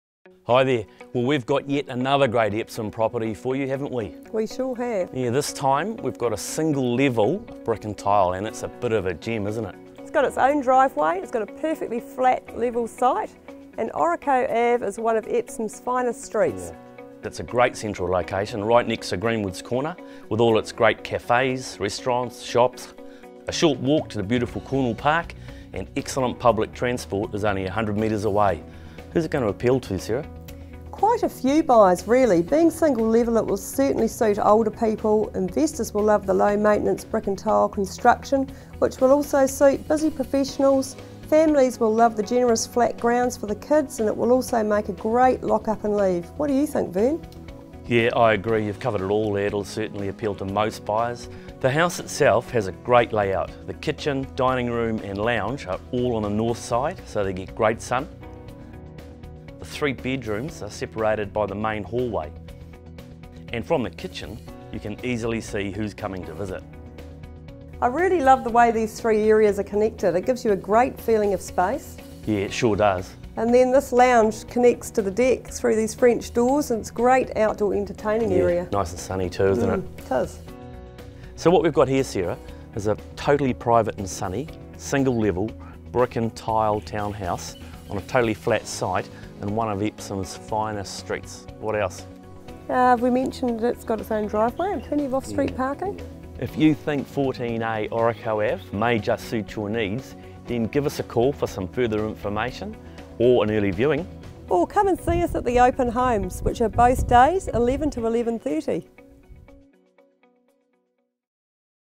SOLD - 14A Orakau Avenue, Epsom - Vern Hines and Sara Knight
GZ Single Level Brick Beauty

So rare we couldn't find any comparable sales in the last 12 months. This fantastic 3 double bedroom single level brick and tile townhouse is on a very generous, totally flat and totally private ...

Property Marketed by: Vern Hines, Mt Eden and Sara Knight, Mt Eden